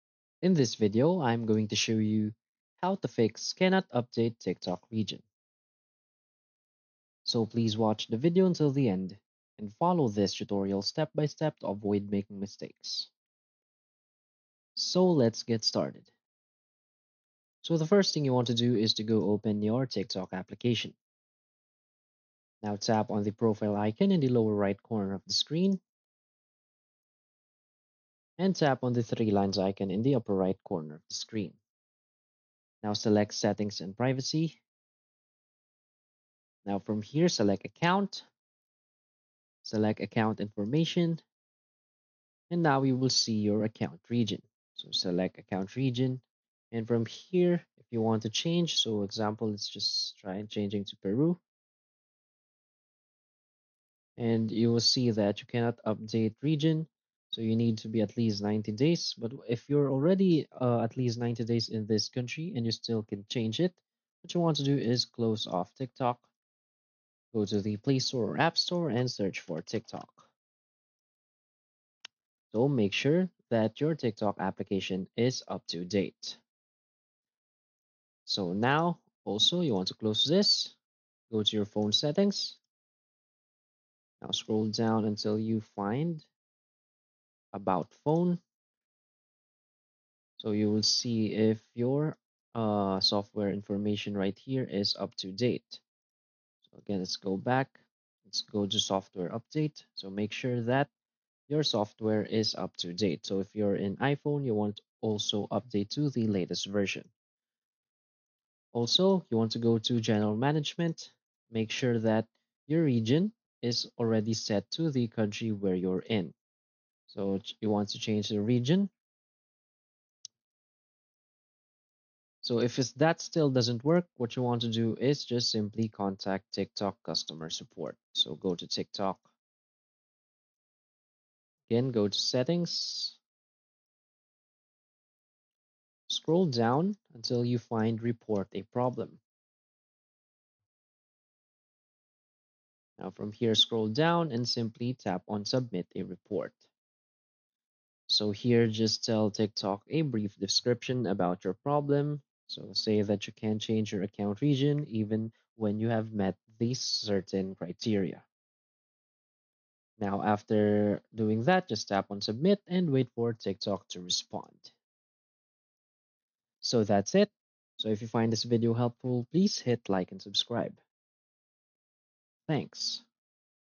How to Fix Cannot Update Tiktok Region 2024
In this video I show you how you can Fix Cannot Update Tiktok Location. Do you wonder how you can Fix Cannot Change Tiktok Region? I'll show you exactly how to do that.